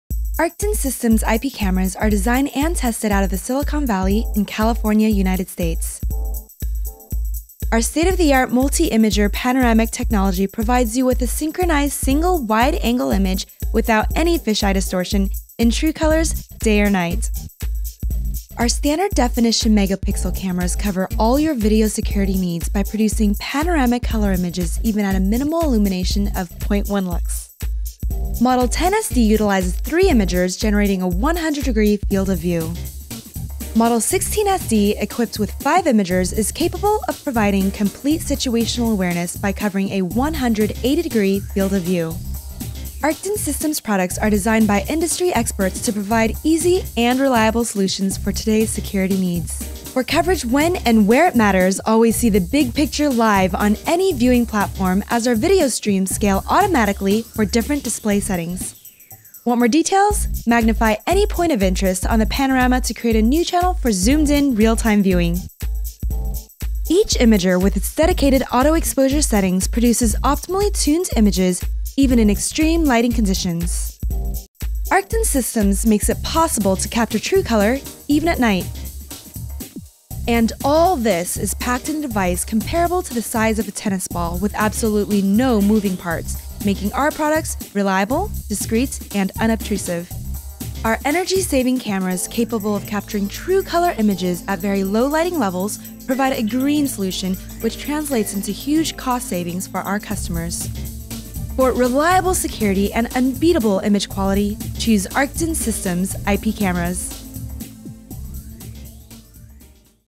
Panoramic camera technology from Arktan Systems
Arktan Systems showcases new panoramic camera technology for adding situational awareness to video surveillance systems.